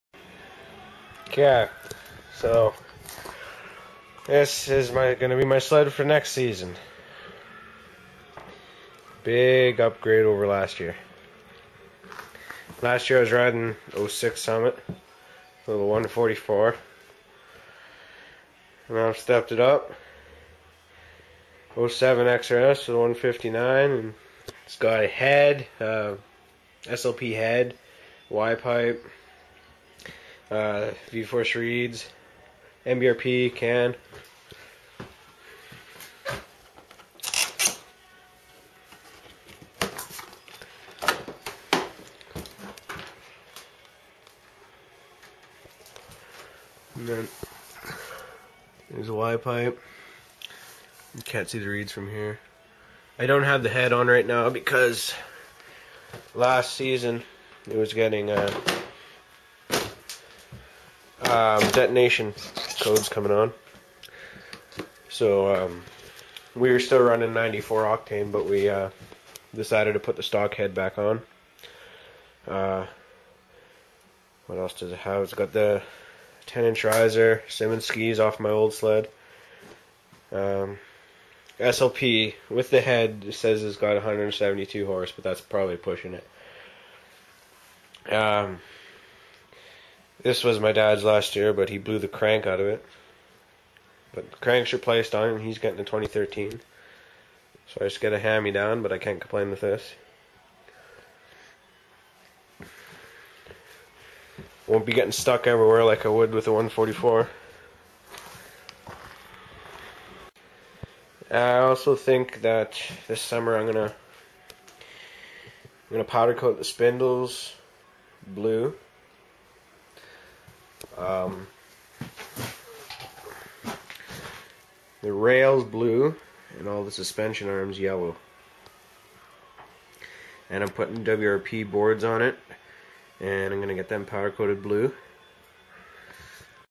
My Current Sled
I just wanted to post this for 2 reasons; 1, I needed to be around my sled for a purpose. 2, I wanted to make clear which sled is mine because I ride alot of different sleds